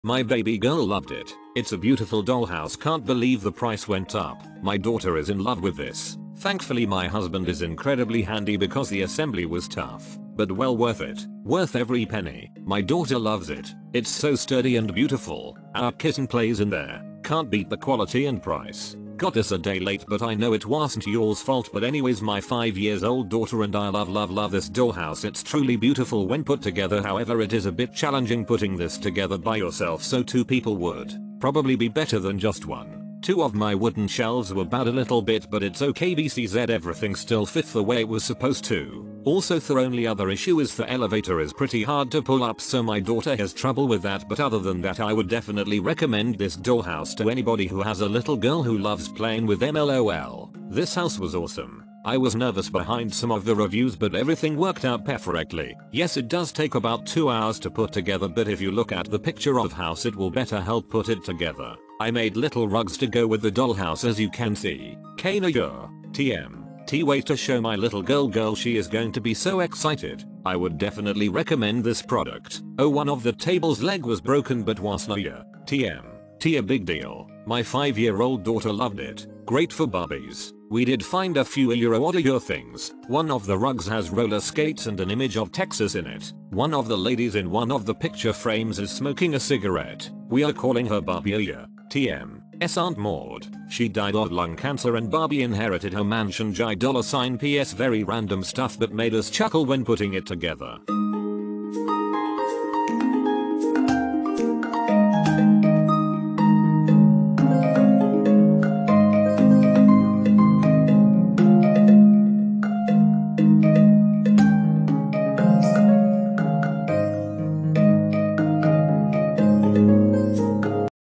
KidKraft Amelia Dollhouse review
Main Features:
by entering your model number.. 3 levels, 4 rooms and a balcony. Gliding elevator. Detailed scrollwork. Smart, sturdy wood construction. Country of Origin: China. Three stories and 13 colorful pieces of furniture. Can accommodate dolls up to 12" tall. Full staircase connects first and second floors. Large enough for multiple children to play at once